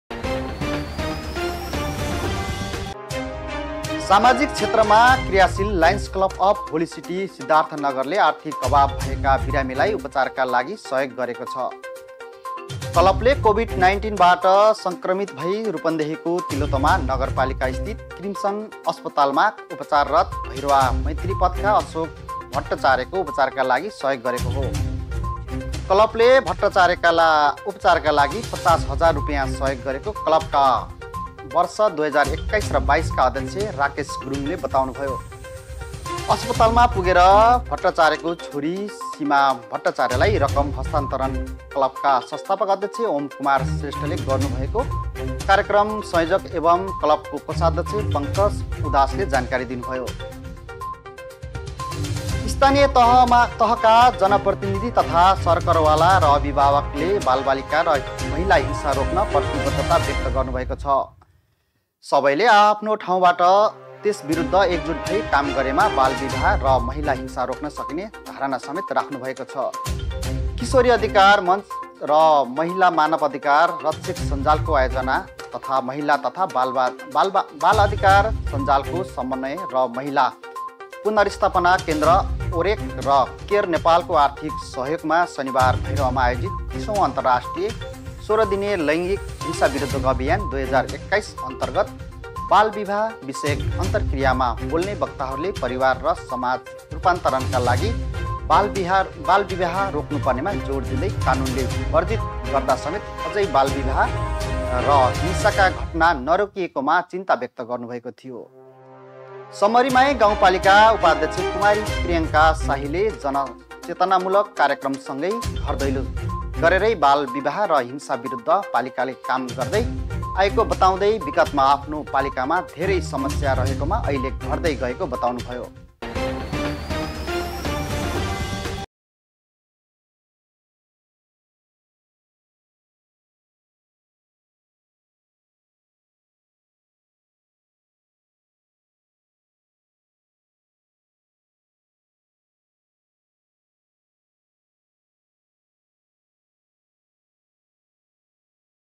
होलिसिटीद्वारा अशोक भट्टचार्यलाई आर्थिक सहयोग II News Ok Live
लायन्स क्लब अफ सिद्धार्थनगर होलिसिटीद्वारा उपचारमा आर्थिक समस्यामा परेका भैरहवा मैत्रीपथका अशोक भट्टचार्यलाई ५० हजार सहयोग ।